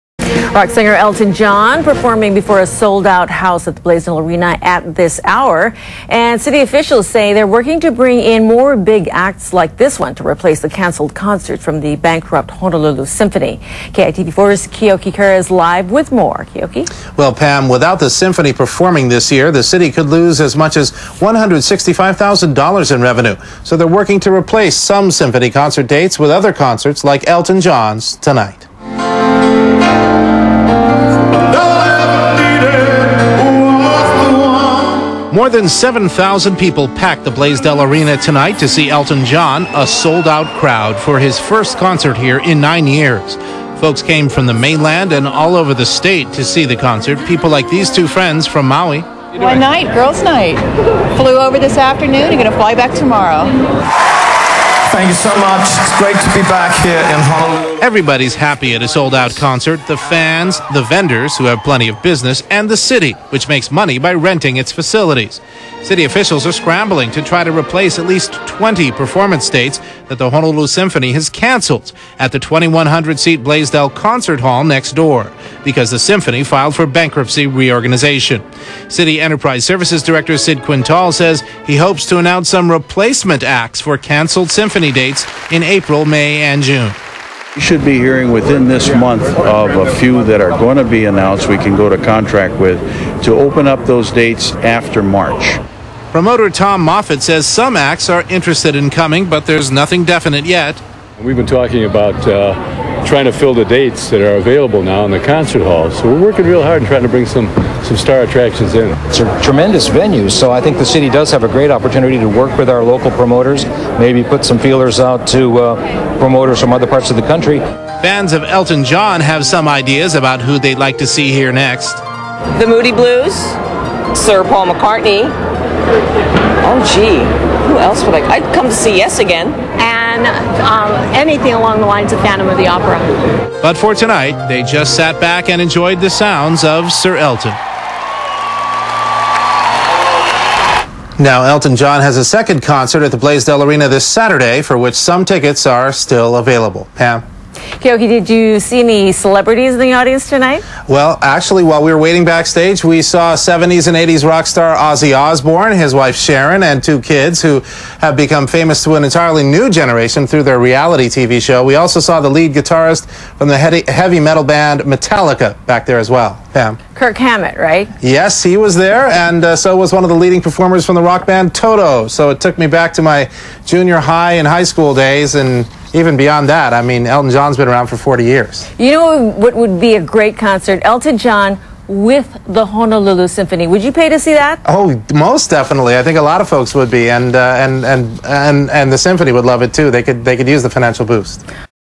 Elton Johns Performance Sells Out
City officials say they're working to bring in more BIG ACTS like Elton John to replace the canceled concerts from the bankrupt Honolulu Symphony. 1/6/2010. For news you need to know,